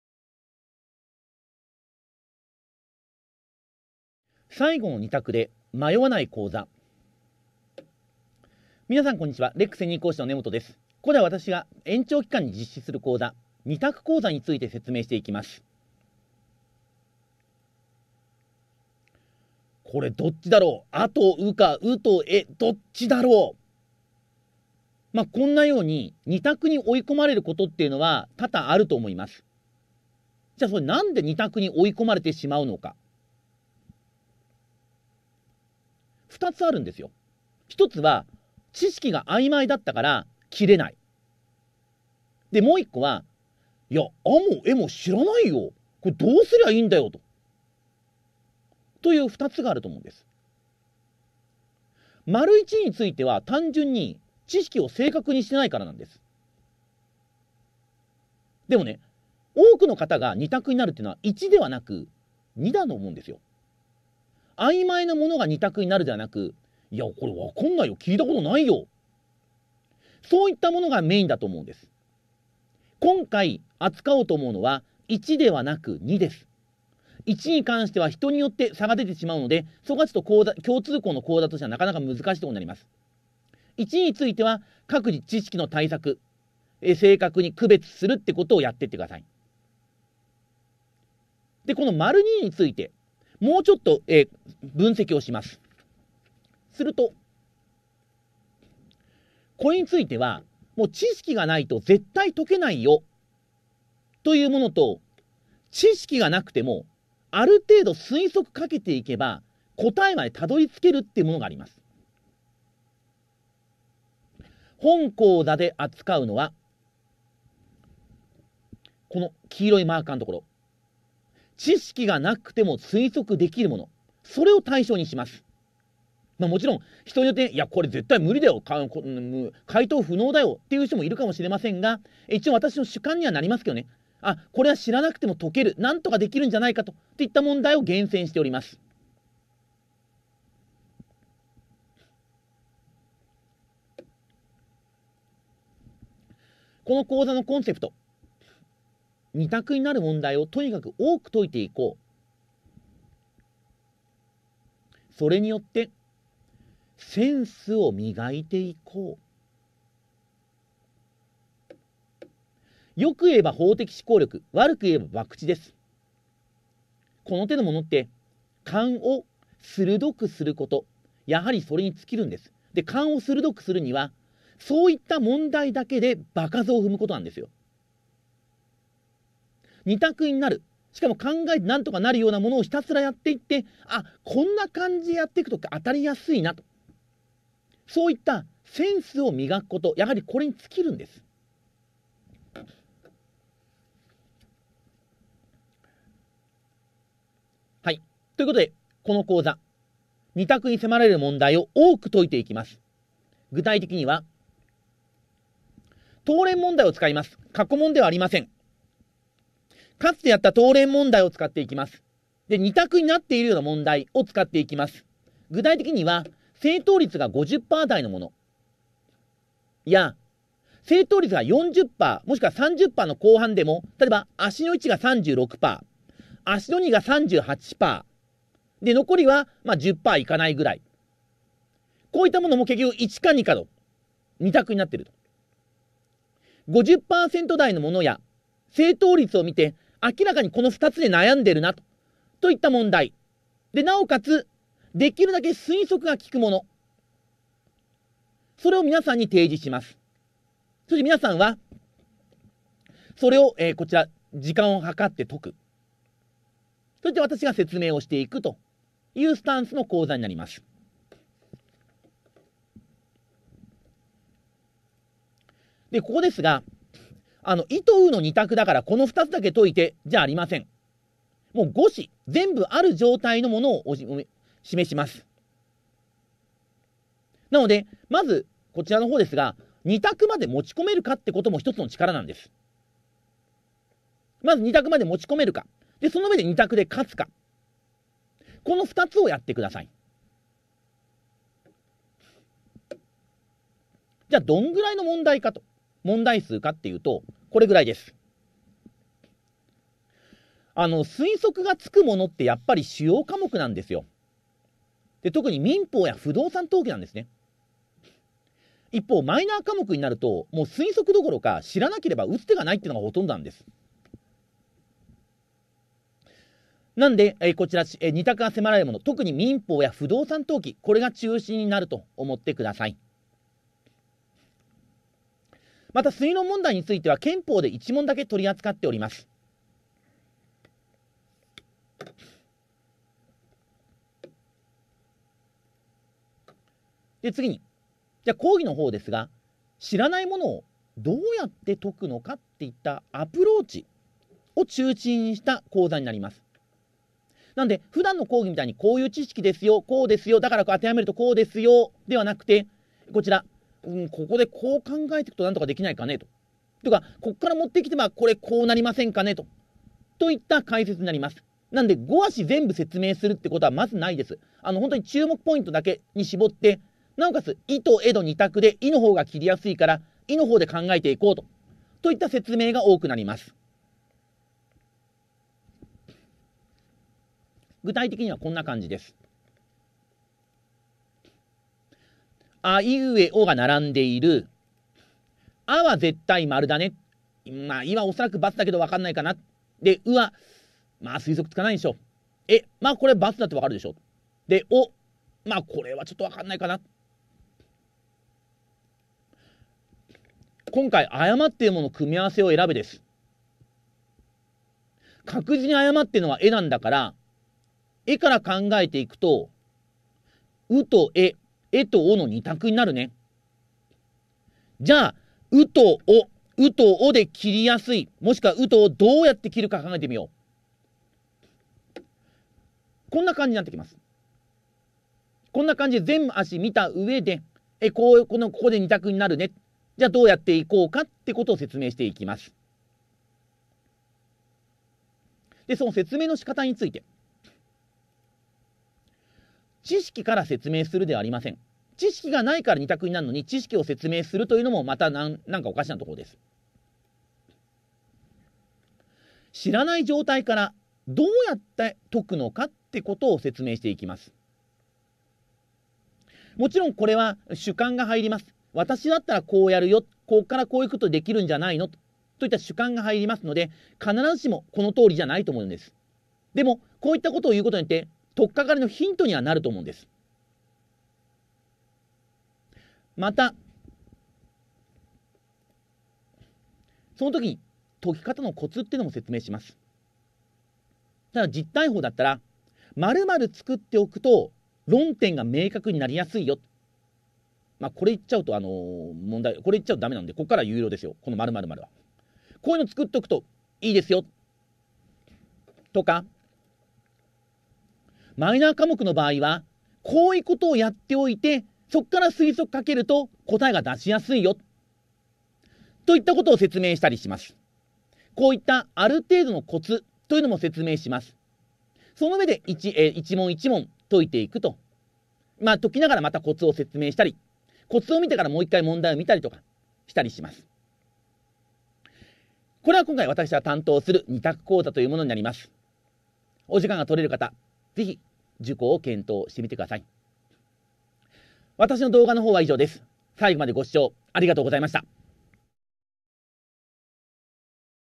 【LEC司法書士】もう迷わない！根本講師による「最後の二択で迷わない講座」
●本試験の延期が発表！LECからの新提案！
「この延長期間で大逆転を目指す」ための講座から、
「この延長期間、実力を維持したい」ペースを
維持する講座までご用意！